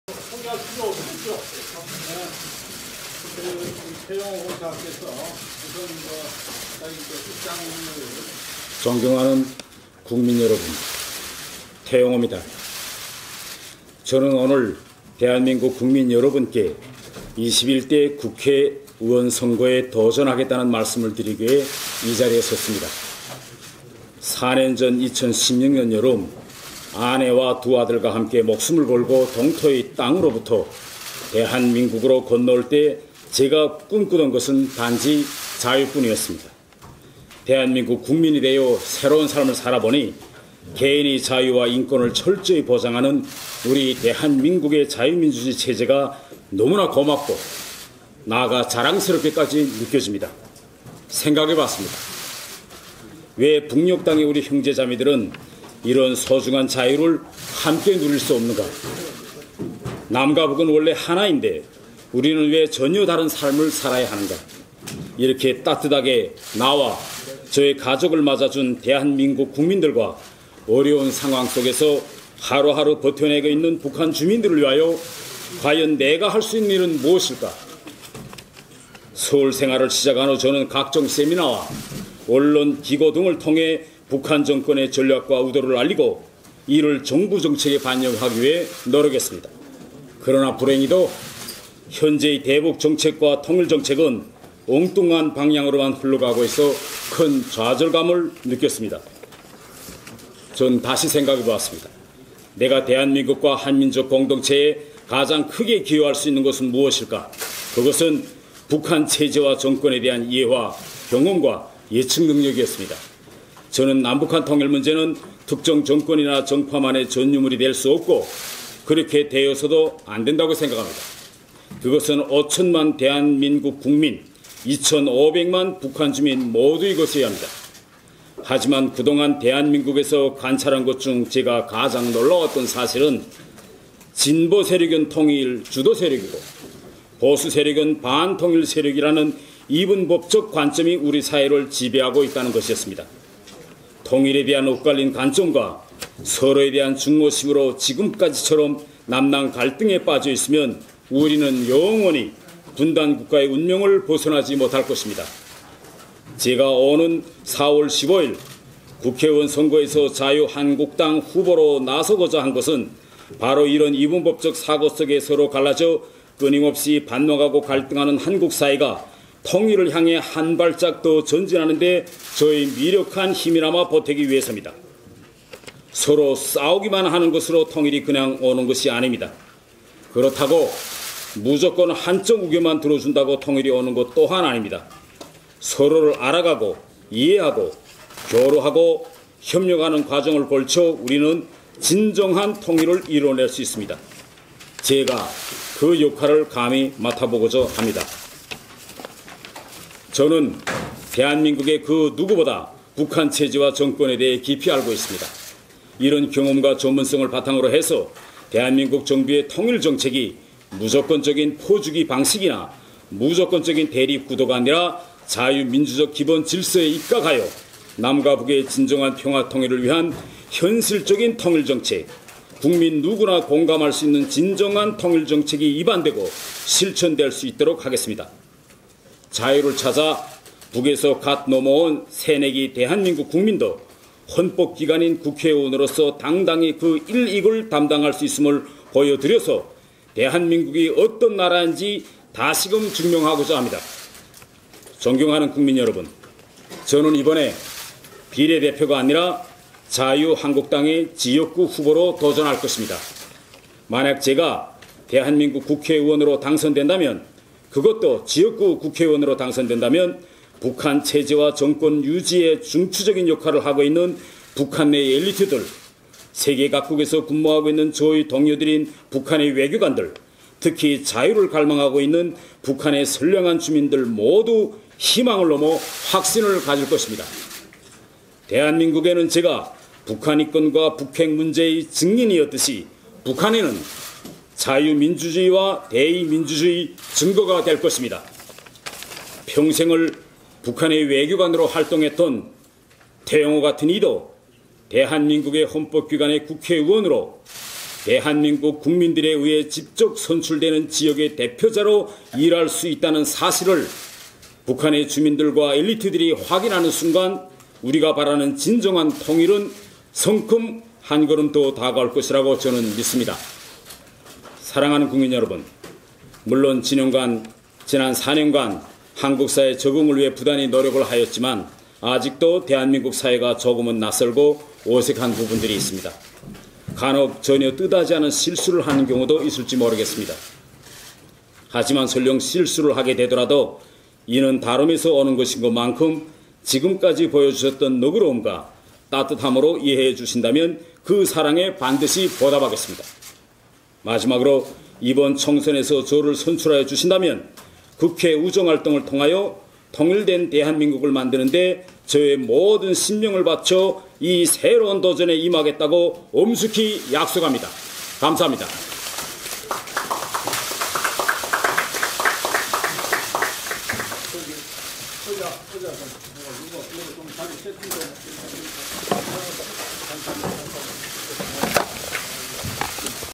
태영호 전 북한공사, 자유한국당 서울 지역구 후보 출마 기자회견 풀영상
국민 여러분, 태영호입니다.
저는 오늘 대한민국 국민 여러분께 제21대 국회의원 선거에 도전하겠다는 말씀을 드리기 위해 이 자리에 섰습니다.
4년 전인 2016년 여름, 아내와 두 아이들과 함께 목숨을 걸고 동토의 땅으로부터 대한민국으로 건너올 때 제가 꿈꾸던 것은 단지 자유뿐이었습니다.
대한민국 국민이 되어 새로운 삶을 살아보니 개인의 자유와 인권을 철저하게 보장하는 우리 대한민국의 자유민주주의 체제가 너무나 고맙고, 나아가 자랑스럽게 느껴집니다.
생각해 봤습니다. 왜 북녘 땅의 우리 형제자매들은 이런 소중한 자유를 함께 누릴 수 없는가. 남과 북은 원래 하나인데 우리는 왜 전혀 다른 삶을 살아야 하는가. 이렇게 따뜻하게 나와 내 가족을 맞아준 대한민국 국민들과 어려운 상황 속에서 하루하루 버텨내고 있는 북한의 주민들을 위하여 내가 할 수 있는 일은 무엇일까.
서울 생활을 시작한 이후, 저는 각종 세미나와 언론 기고 등을 통해 북한 정권의 전략과 의도를 알리고, 이를 정부 정책에 반영하기 위해 노력해 왔습니다. 그러나 불행히도 현재의 대북 정책과 통일 정책은 엉뚱한 방향으로만 흘러가고만 있어 큰 좌절감을 느꼈습니다.
다시 생각해 봤습니다. 내가 대한민국과 한민족공동체에 가장 크게 기여할 수 있는 것은 무엇일까. 그것은 북한 체제와 정권에 대한 이해와 경험과 예측 능력이었습니다.
저는 남북한 통일 문제는 특정 정권이나 정파만의 전유물이 될 수 없고 그렇게 되어서도 안 된다고 생각합니다. 그것은 5천만 대한민국 국민, 2천 5백만 북한 주민 모두의 것이어야 합니다. 하지만, 그동안 대한민국에서 관찰한 것 중 가장 놀라웠던 사실은, 진보세력은 통일주도세력이고 보수세력은 반통일세력이라는 이분법적 관점이 우리 사회를 지배하고 있다는 것이었습니다.
통일에 대한 엇갈린 관점과 서로에 대한 증오심으로 지금까지처럼 남남 갈등에 빠져 있으면 우리는 영원히 분단국가의 운명을 벗어나지 못할 것입니다.
제가 오는 4월15일 국회의원 선거에서 자유한국당 후보로 나서고자 한 것은, 바로 이런 이분법적 사고 속에 서로 갈라져 끊임없이 반목하고 갈등하는 한국 사회가 통일을 향해 한 발짝 더 전진하는데 저의 미력한 힘이나마 보태기 위해서입니다.
서로 싸우기만 하는 것으로 통일이 그냥 오는 것이 아닙니다.
그렇다고 무조건 한쪽 의견만 들어준다고 통일이 오는 것 또한 아닙니다.
서로를 알아가고 이해하고 교류하고 협력하는 과정을 거쳐 우리는 진정한 통일을 이뤄낼 수 있습니다. 제가 그 역할을 감히 맡아보고자 합니다.
저는 대한민국의 그 어느 누구보다 북한 체제와 정권에 대해 깊이 알고 있습니다. 이런 경험과 전문성을 바탕으로 해서 대한민국 정부의 통일 정책이 무조건적인 퍼주기 방식이나 무조건적인 대립 구도가 아니라, 자유민주적 기본질서에 입각하여 남과 북의 진정한 평화통일을 위한 현실적인 통일정책, 국민 누구나 공감할 수 있는 진정한 통일정책이 입안되고 실천될 수 있도록 하겠습니다.
자유를 찾아 북에서 갓 넘어온 새내기 대한민국 국민도 헌법기관인 국회의원으로서 당당히 그 일익을 담당할 수 있음을 보여드려서 대한민국이 어떤 나라인지 다시금 증명하고자 합니다.
존경하는 국민 여러분,
저는 이번 총선에 비례대표가 아니라 자유한국당의 지역구 후보로 도전할 것입니다.
만약 제가 대한민국 국회의원으로 당선된다면, 그것도 지역구 국회의원으로 당선된다면, 북한체제와 정권의 유지에 중추적인 역할을 하고 있는 북한 내의 엘리트들, 세계 각국에서 근무하고 있는, 저의 옛 동료들인 북한의 외교관들, 특히 자유를 갈망하고 있는 북한의 선량한 주민들 모두, 희망을 넘어 확신을 가질 것입니다.
대한민국에는 제가 북한인권과 북핵문제의 증인이었듯이 북한에는 자유민주주의와 대의민주주의의 증거가 될 것입니다.
평생을 북한의 외교관으로 활동했던 태영호 같은 이도 대한민국의 헌법기관인 국회의원으로, 대한민국 국민들에 의해 직접 선출되는 지역의 대표자로 일할 수 있다는 사실을 북한의 주민들과 엘리트들이 확인하는 순간, 우리가 바라는 진정한 통일은 성큼 한 걸음 더 다가올 것이라고 저는 믿습니다.
사랑하는 국민 여러분,
물론, 지난 4년간 한국 사회 적응을 위해 부단한 노력을 하였지만, 아직도 대한민국 사회가 조금은 낯설고 어색한 부분들이 있습니다. 간혹 전혀 뜻하지 않은 실수를 하는 경우도 있을지 모르겠습니다. 하지만, 설령 실수를 하게 되더라도 이는 다름에서 오는 것이니만큼 지금까지 보여주셨던 너그러움과 따뜻함으로 이해해 주신다면 그 사랑에 반드시 보답하겠습니다.
마지막으로 이번 총선에서 저를 선출하여 주신다면, 국회 의정활동을 통하여 ‘통일된 대한민국’을 만드는 데 저의 모든 신명을 바쳐 이 새로운 도전에 임하겠다고 엄숙히 약속합니다.